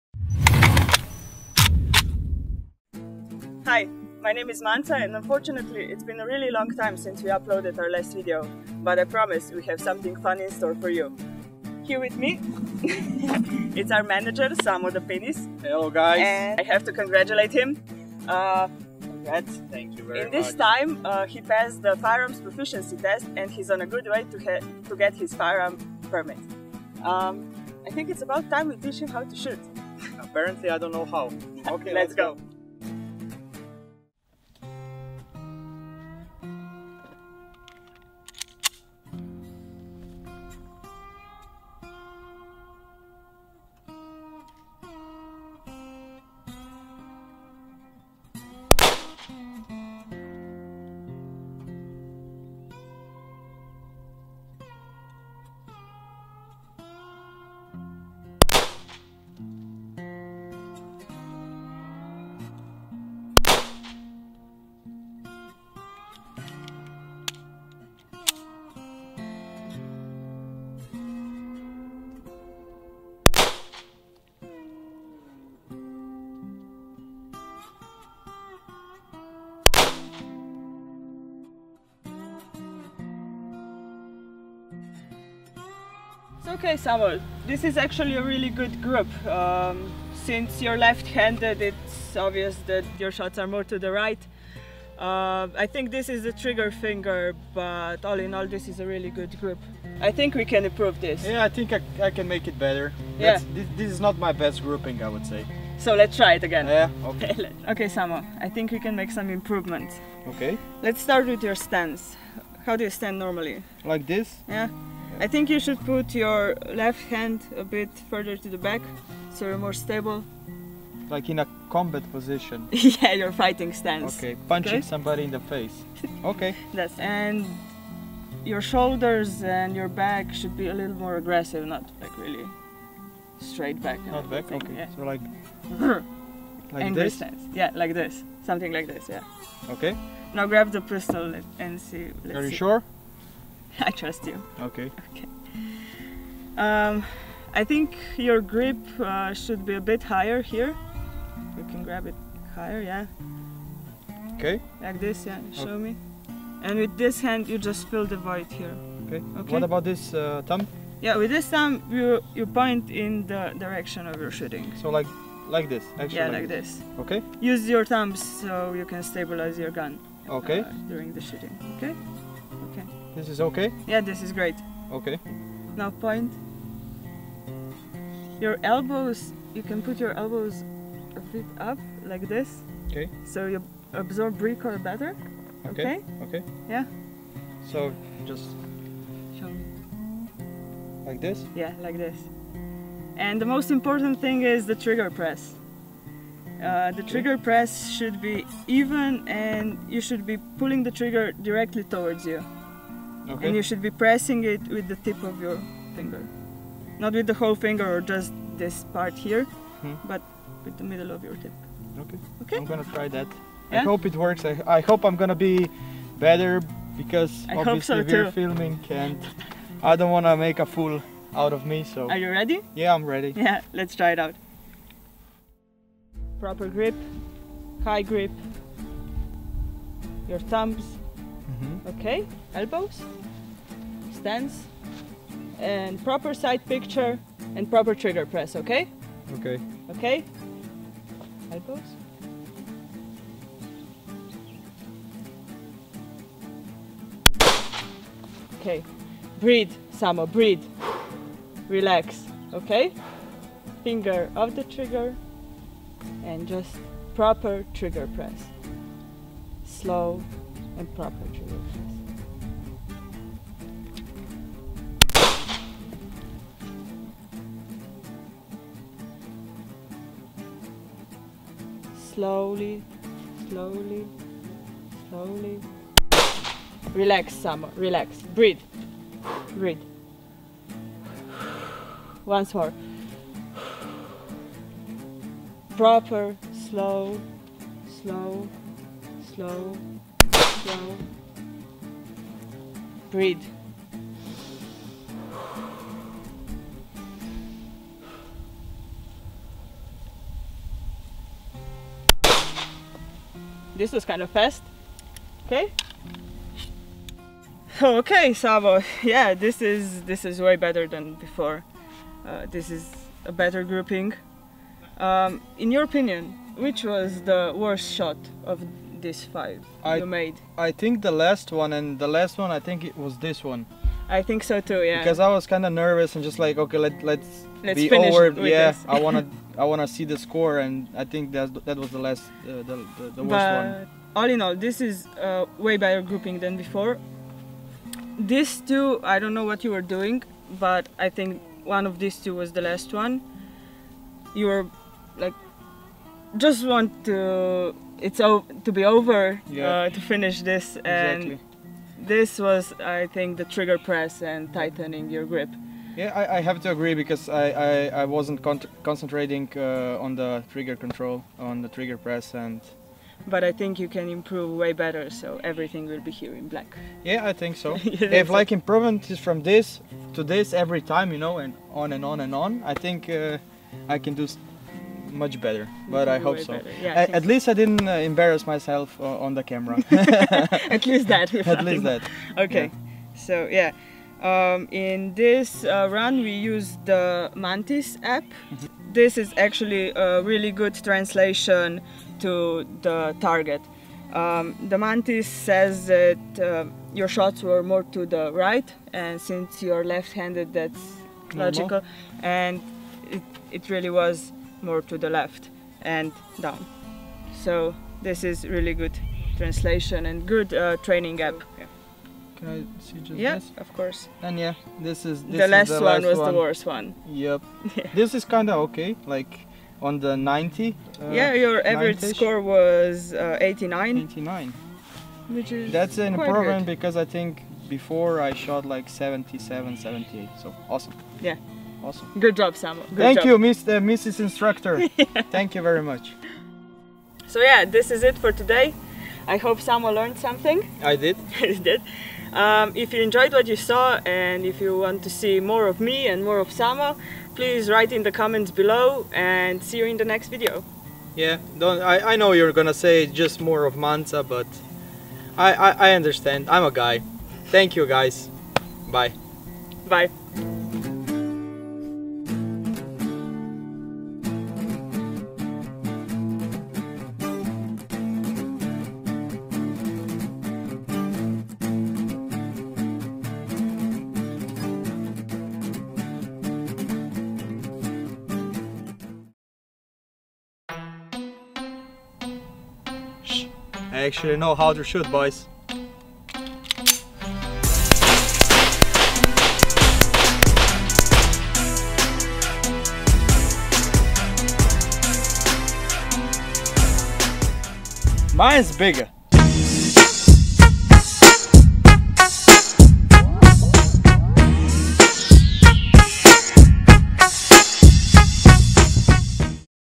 Manca's Shooting instructions
Manca gives Samo a couple of tips on how to shoot better with a handgun.